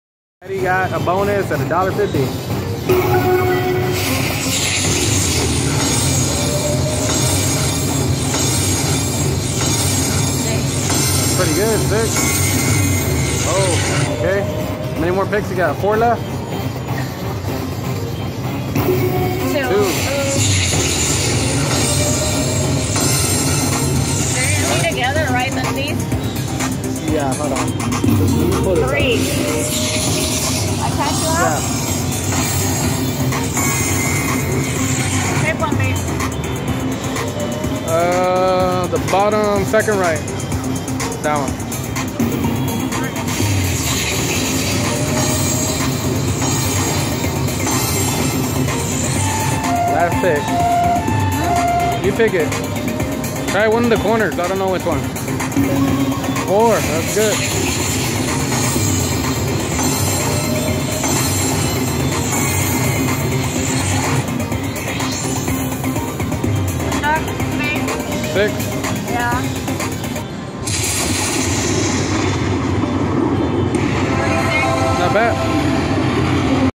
BLACKOUT SLOT MACHINE AT KICKAPOO bonus
Did 150 bet and won a good amount.